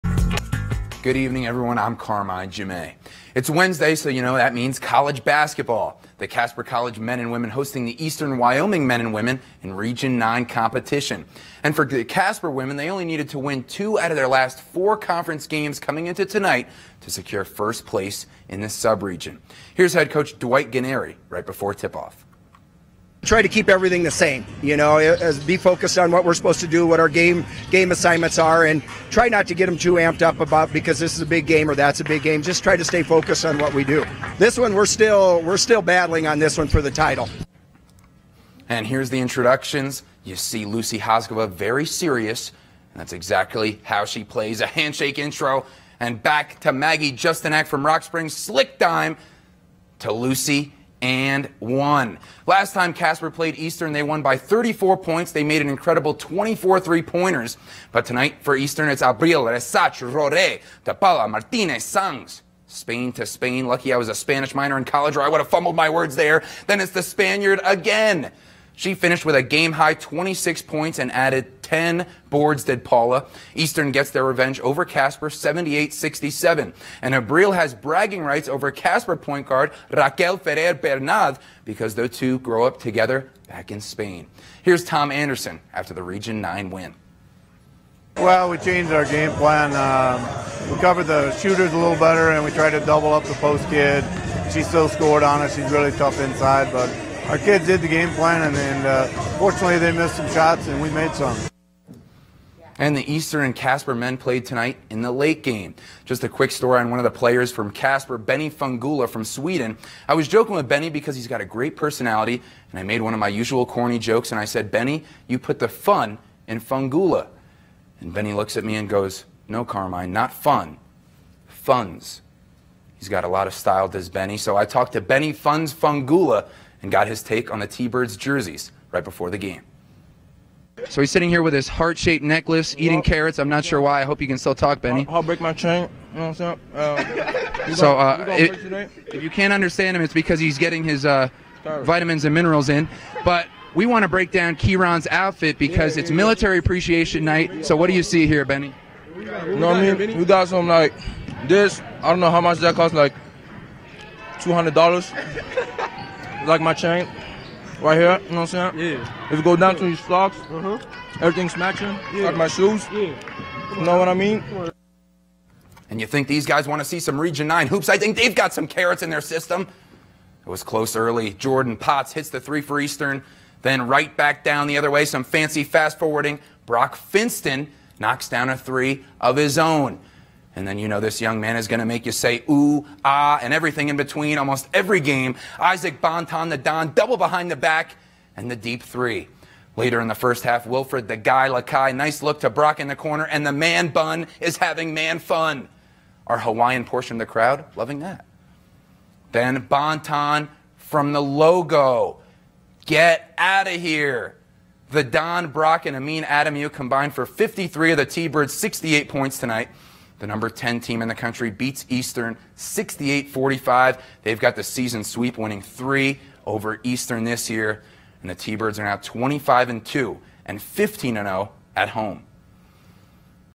Casper College Men's and Women's Basketball Hosts Eastern Wyoming
Highlights and reaction from an exciting Region 9 doubleheader tonight in The Swede at Casper College!